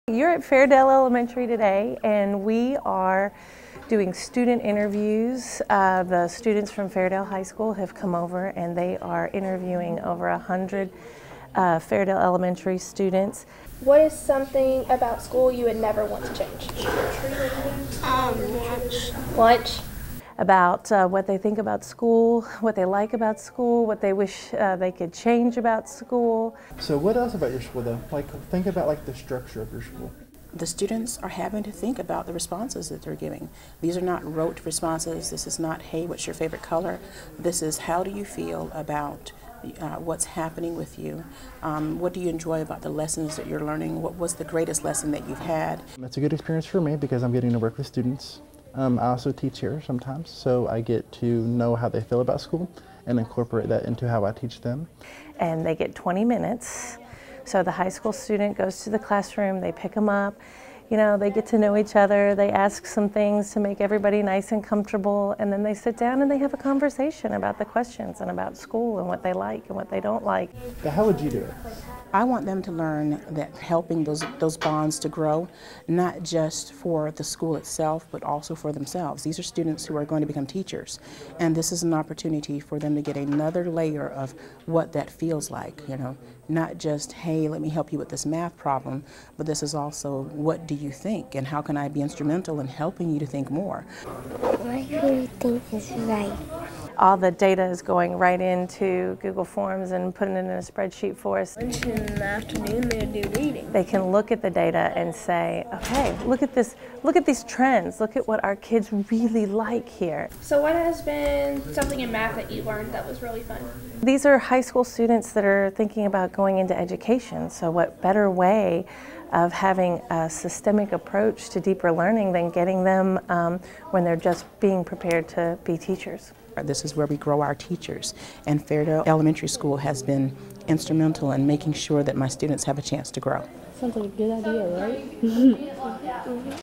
Fairdale Elementary & High Schools -- Deeper Learning
Students from Fairdale High School's Education Academy interviewed Fairdale Elementary students as part of a Deeper Learning project.